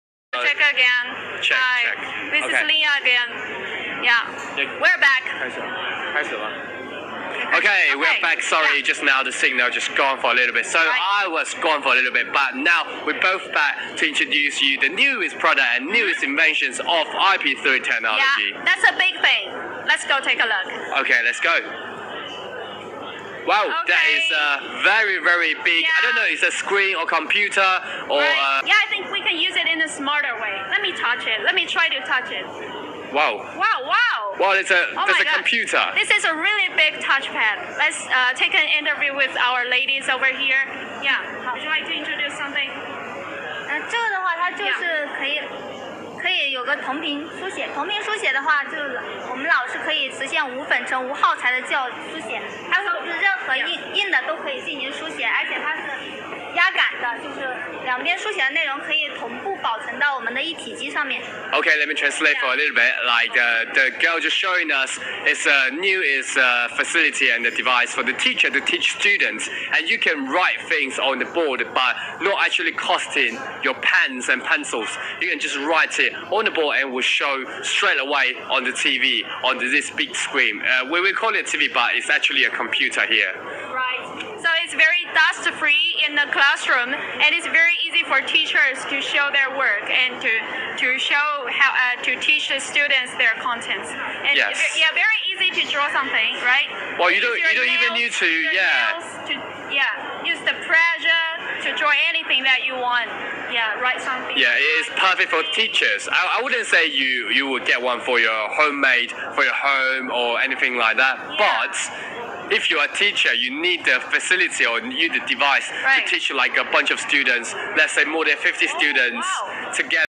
SCT INTERACTIVE LCD BLACKBOARD INTRODUCTION
It is SCT double backboard products, it is used for intelligent teaching, widely used in colleges, secondary and primary schools, with teaching tools for many subjects, it can make abstract contents to be visual.

Interconnection integration, abandoning a series of complex operation, allowing teachers to quickly master the uses, to switch the functions of OC Smart full screen product in short time.

Energy saving and eyes protection
It is ultra-low standby power consumption, adapting intelligent light-sensing brightness adjustment system, applying etching anti-glare technology, achieve true anti-glare protection of eyes.

Synchronous writing
Through the infrared touch technology, the LCD writing Pad and Interactive blackboard are  seamlessly connected, the contents written by teacher on the LCD writing Pad can be synchronously displayed on the screen of blackboard， which  can be shared and stored by scanning the bar code shown.

Environment protection without dust
Through the interconnection between the LCD writing Pad and intelligent blackboard, it can replace the traditional blackboard, eliminating the chalk dust, achieving the multi-scene teaching mode under chalk dust-free environmental protection.

Shortcut buttons on two sides
Customized patent shortcut buttons for teacher to achieve one-click sharing of content, make teaching easily and efficiently.